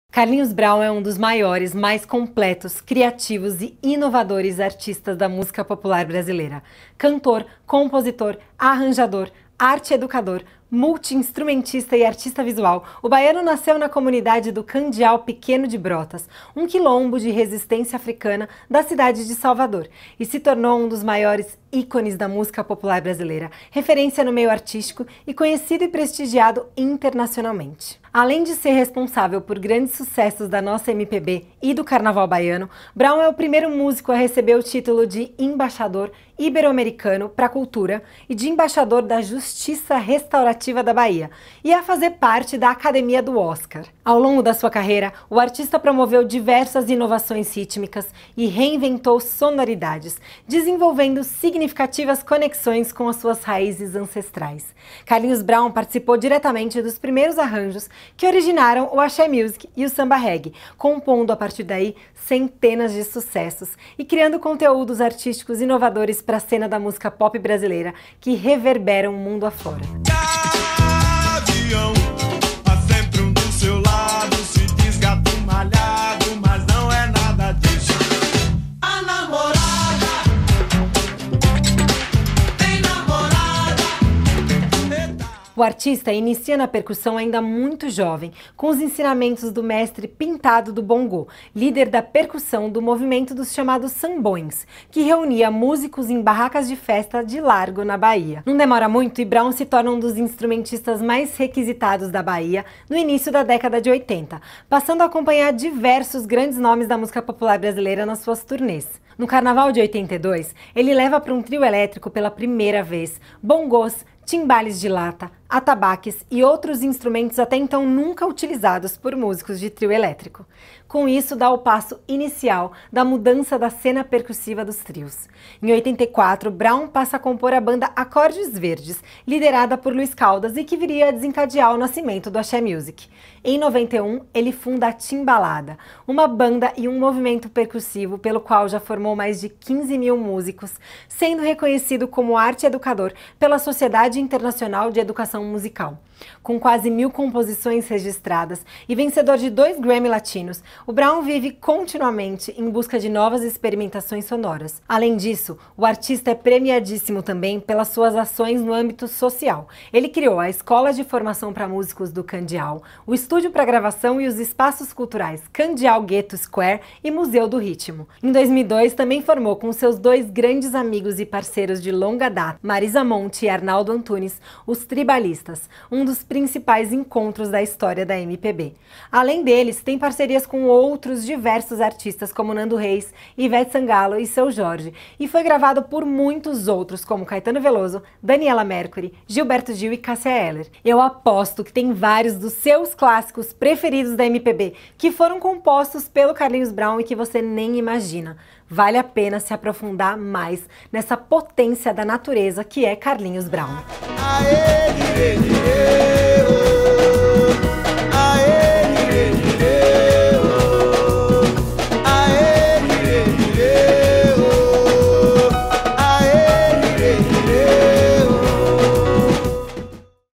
Acervo MPB | Carlinhos Brown
Carlinhos Brown é um dos maiores, mais completos, criativos e inovadores artistas da música popular brasileira.

Cantor, compositor, arranjador, arte-educador, multi-instrumentista e artista visual, o baiano nasceu na comunidade do Candeal Pequeno de Brotas, um quilombo de resistência africana da cidade de Salvador, e se tornou um dos maiores ícones da música brasileira, referência no meio artístico, conhecido e prestigiado internacionalmente.

E a Novabrasil produziu esse podcast contando diversas coisas dessa grandiosa carreira que o Carlinhos Brown construiu. Além deste conteúdo, temos uma matéria completinha lá no nosso site.

Nos acompanhe pelo Instagram: @novabrasilfm